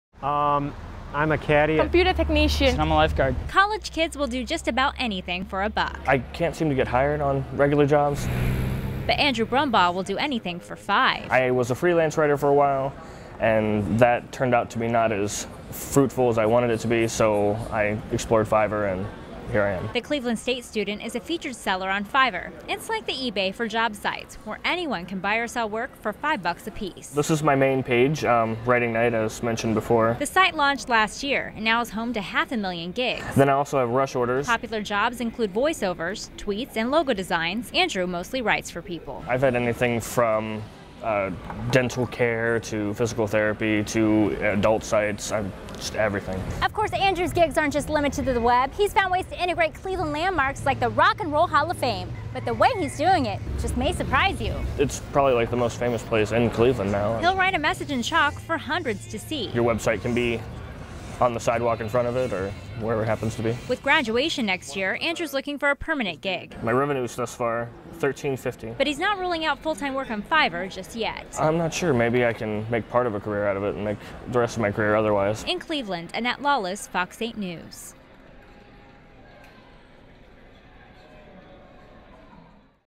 College student makes living on $5 jobs
It can be difficult to find work in today's economy, so a local student turned to the internet. Fox 8's Annette Lawless shows us a job that's earned him hundreds -- $5 at a time.